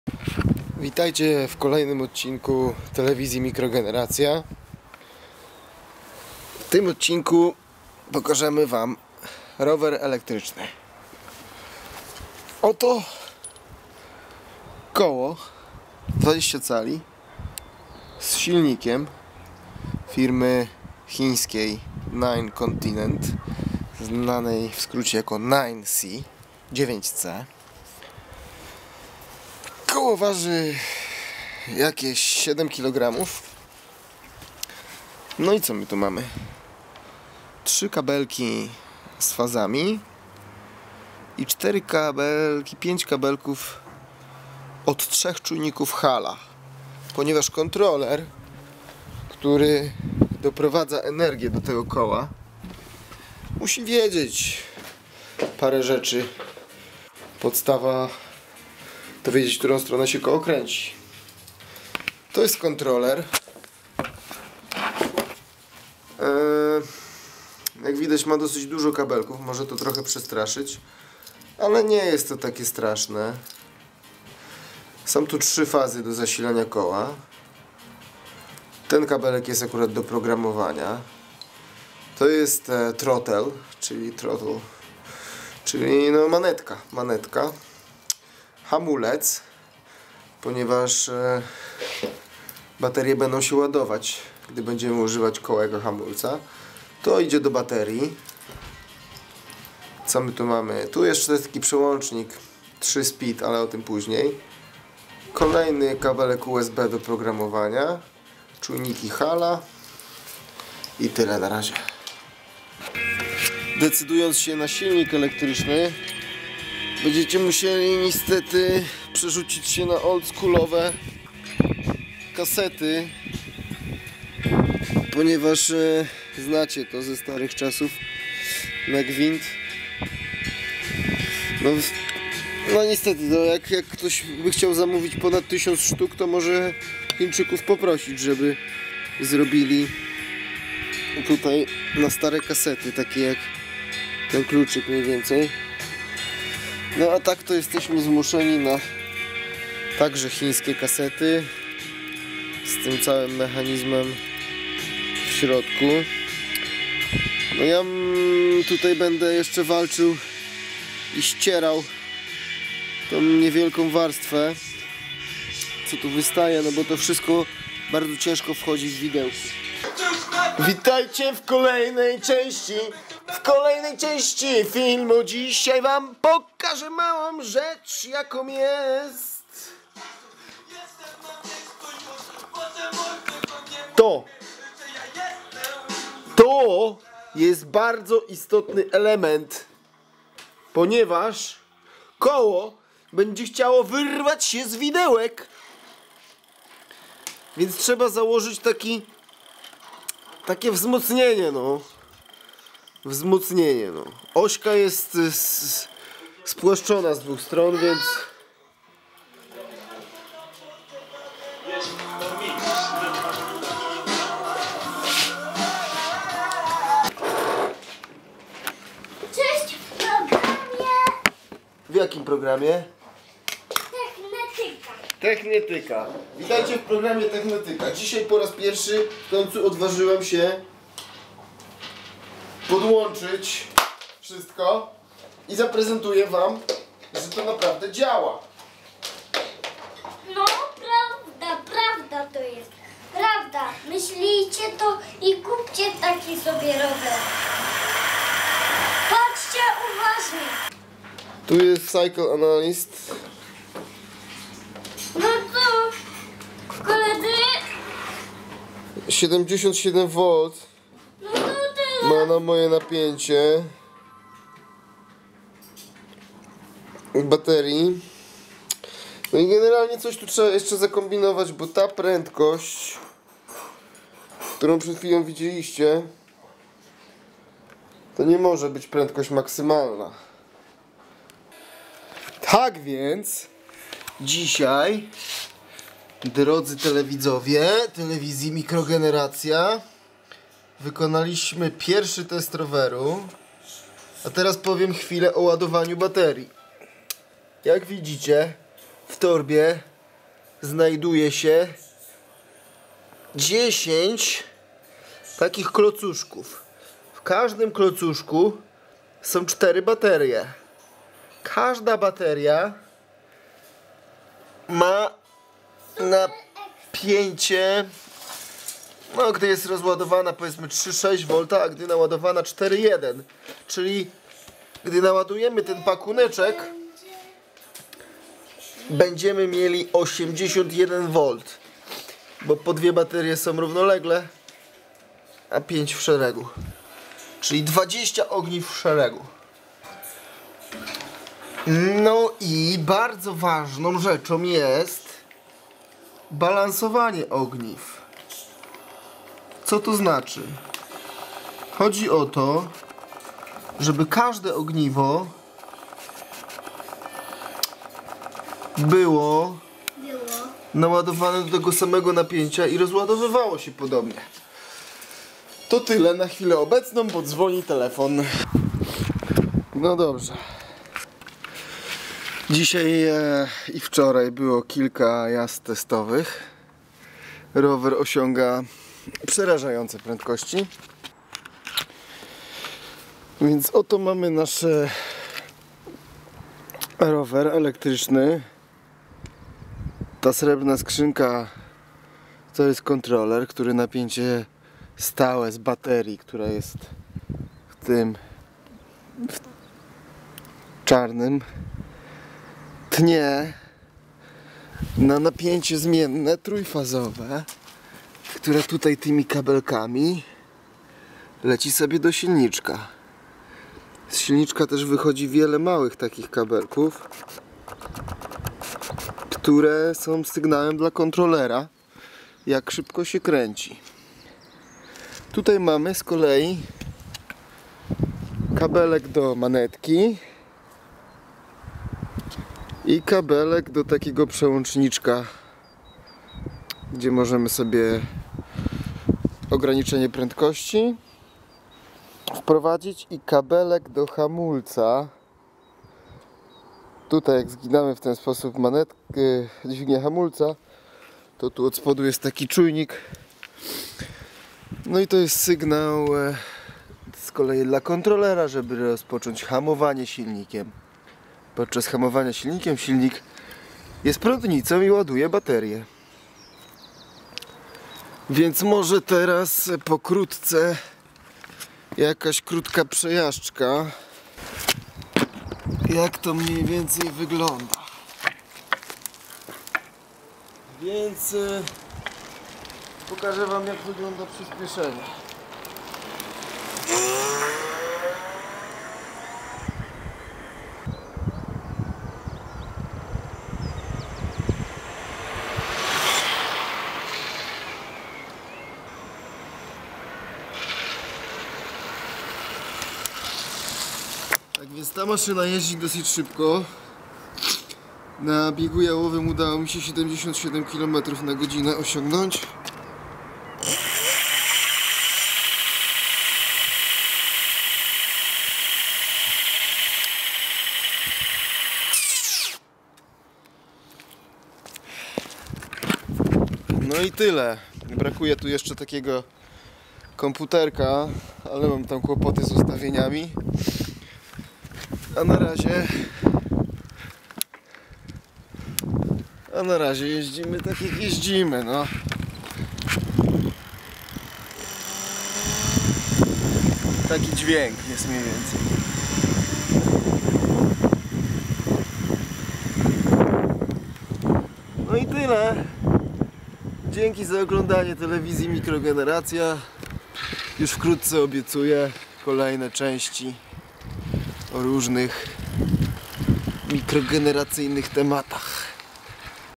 rower elektryczny 66km/h max speed, kola 20 cali, barerie lipo 80V, 45A, regen brake, 3kW mocy !
o budowie roweru elektrycznego na ramie roweru Dahon Jetstream,
pytania ślijcie przez stronke Marek